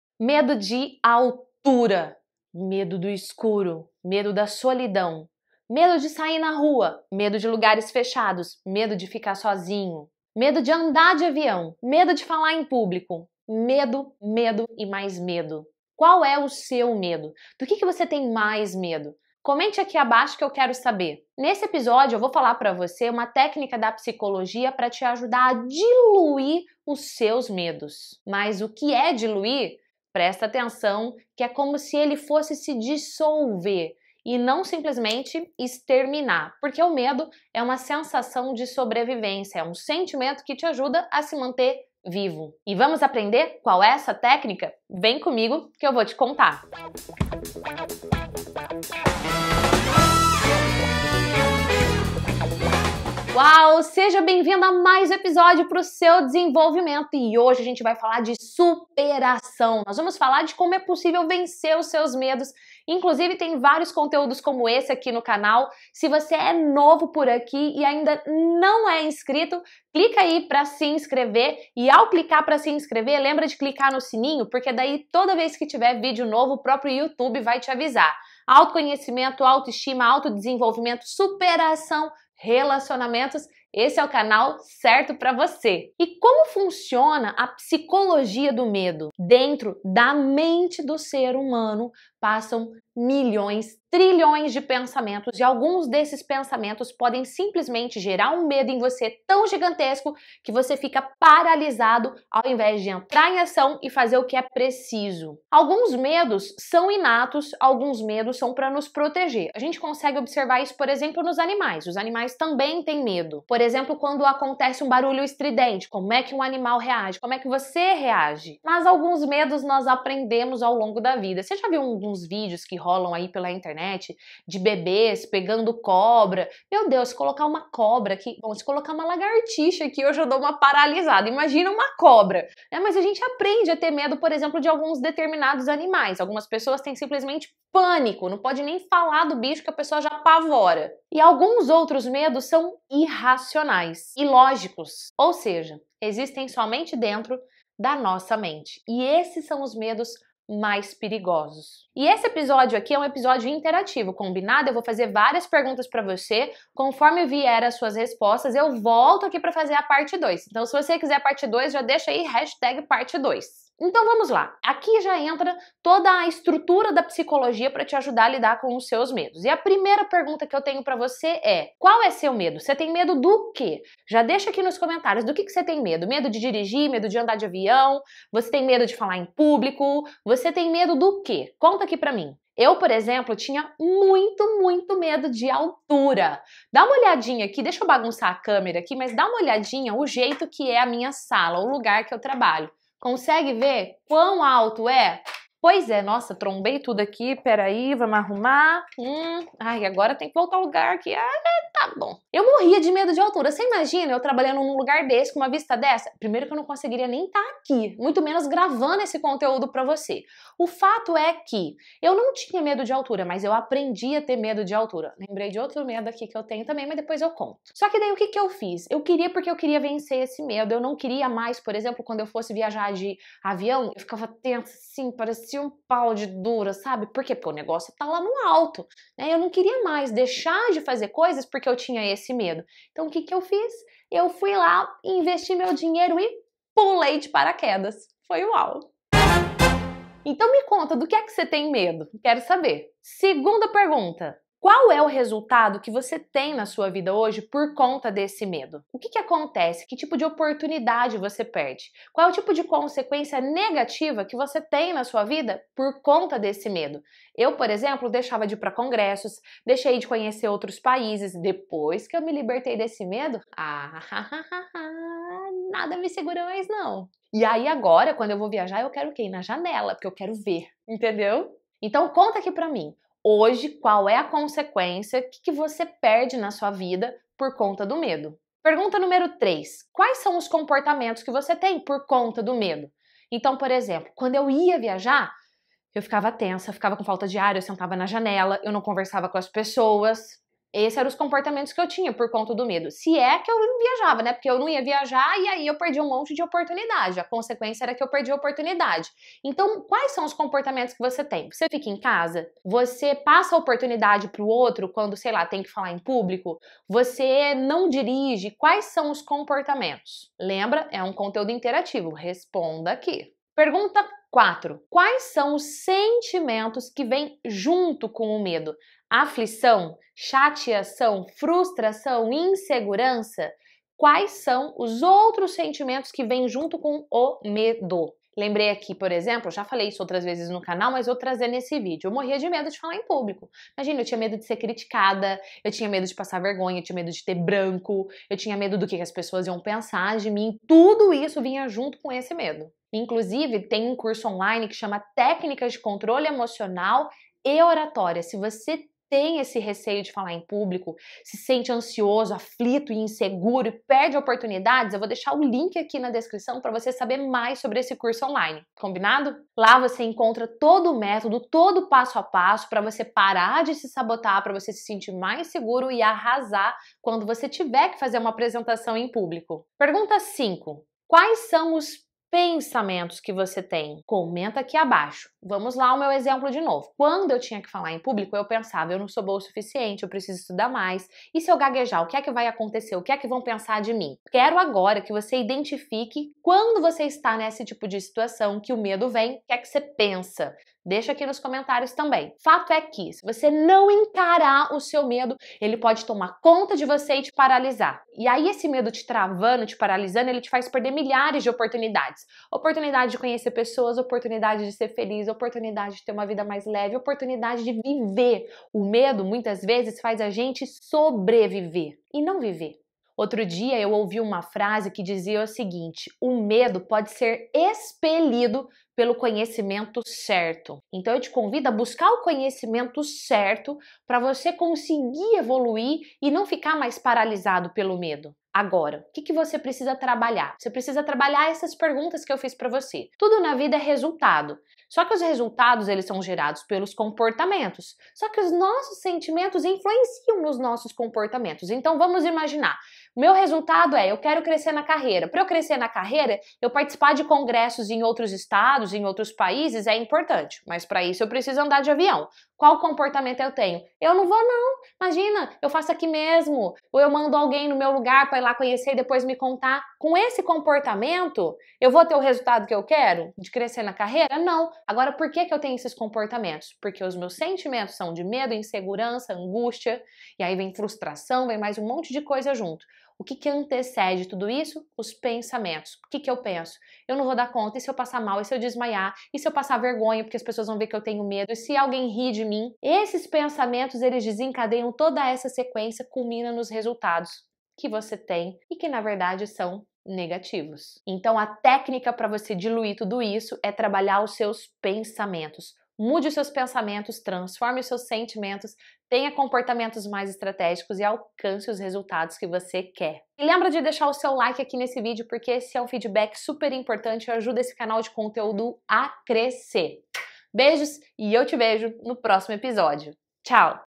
Como vencer o medo - Técnica simples para diluir os seus medos
Como vencer o medo - Técnica simples para diluir os seus medos Como vencer o medo? Você já se perguntou isso? No vídeo de hoje eu apresento a vocês uma Técnica simples para diluir os seus medos e como fazer sua mente superar seus medos. Você quer descobrir como superar os medos para construir auto estima? Ou, como superar o medo de falar em publico? Então, esse vídeo é para você! Pode ser que você me pergunte, "Gi, como superar o medo?" ou "Gi, é possível perder o medo?" e eu te respondo nesse vídeo que SIM, é possível! Vou te ensinar como vencer o medo e a ansiedade.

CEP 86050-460